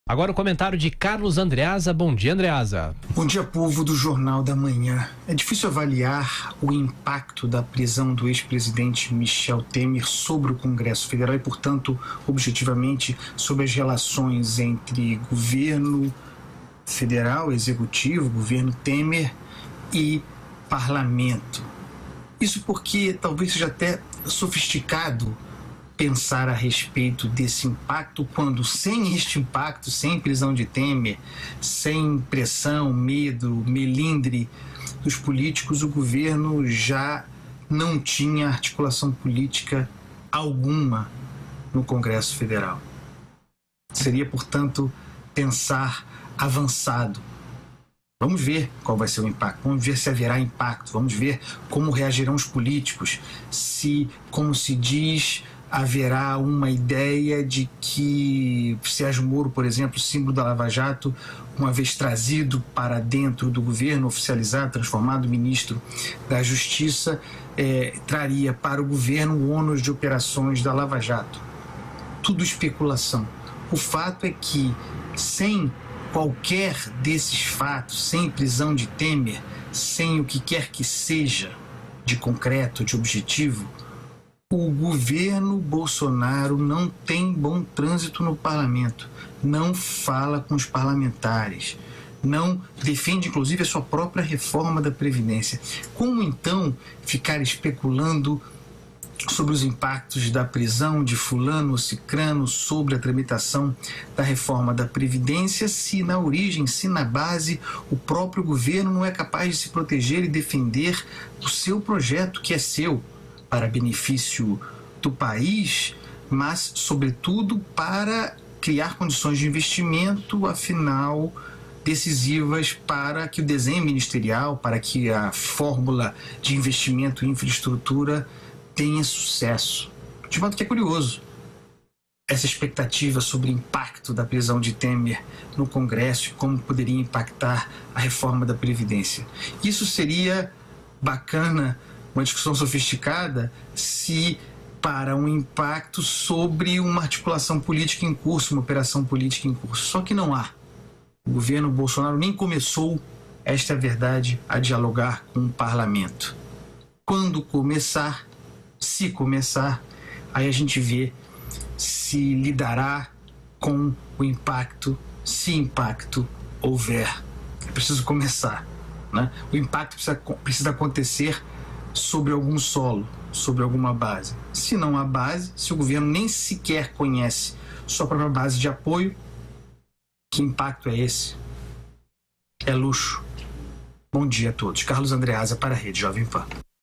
Não há como especular impacto da prisão de Temer na reforma da Previdência
Para Carlos Andreazza, não há como especular impacto da prisão de Temer na reforma da Previdência.